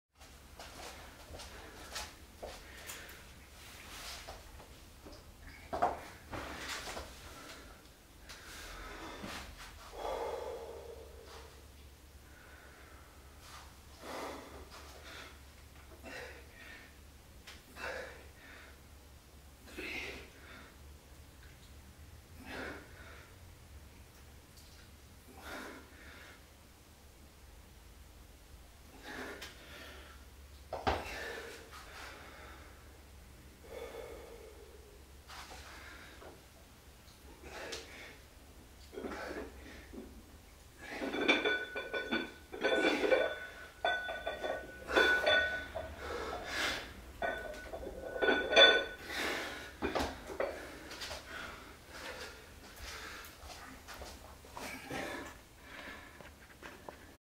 22,5 kilogram concentration dumbbell bicep curls for 6 reps
22,5 kilogram concentration dumbbell bicep curls for 6 reps